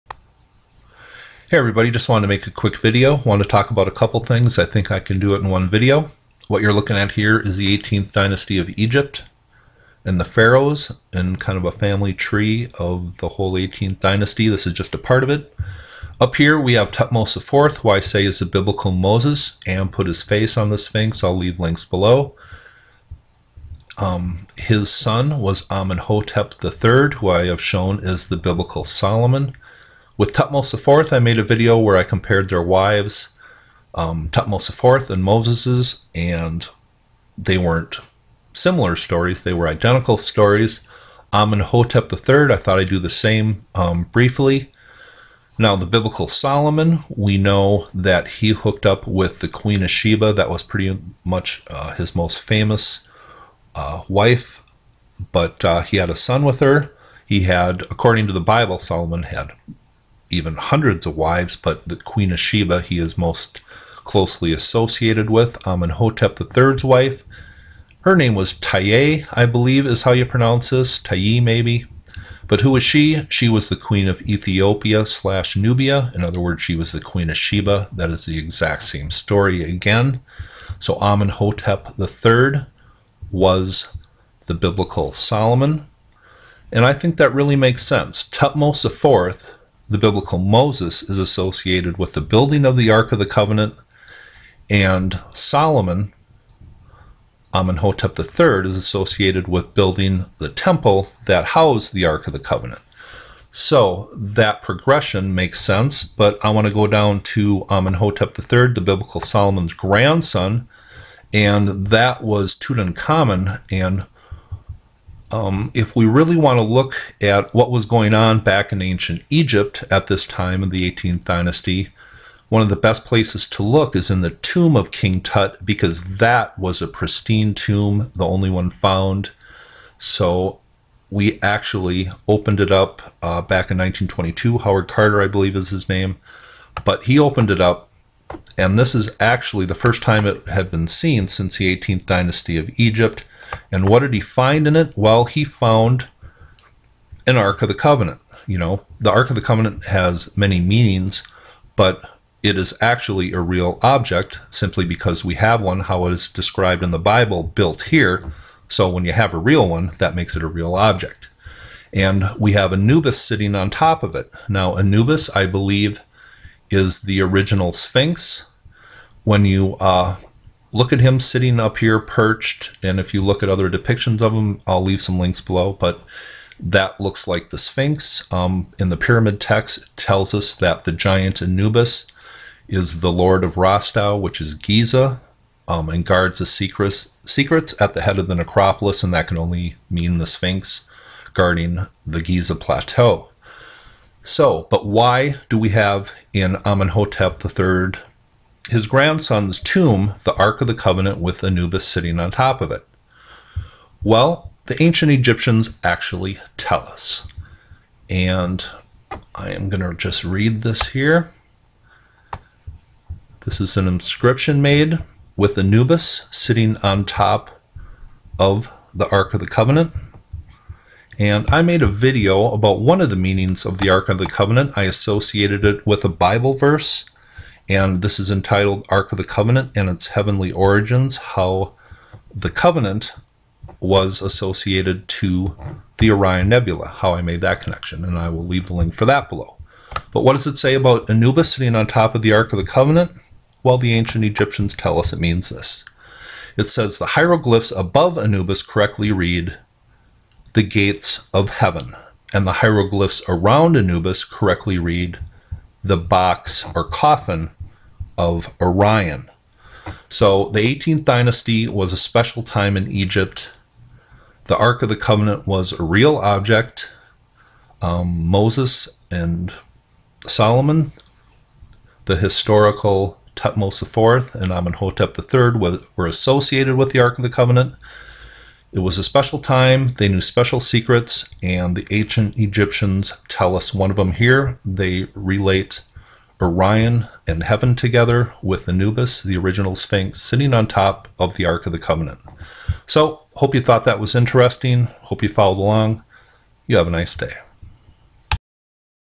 Anubis, The Ark & The Gates Of Heaven
The 18th dynasty knew special things. Lets look at what they tell us. The "Coffin of Orion" sat upon by the original Sphinx.

Photo of Anubis in King Tut's tomb from Harry Burton